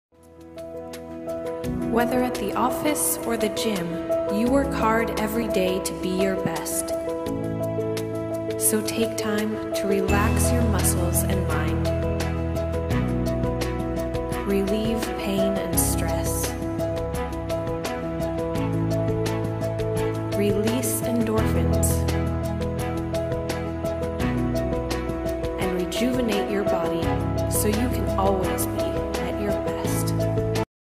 Acupressure Mat
It’s the perfect way to start or end your day! Daily use of the Acupressure Mat stimulates blood circulation for muscle recovery and releases endorphins to naturally reduce aches and tension. Once accustomed to the sensation, the points help muscles relax to promote better rest and destress at the end of the day. Lie or stand on it in the morning to get the blood moving to help naturally energize and wake you up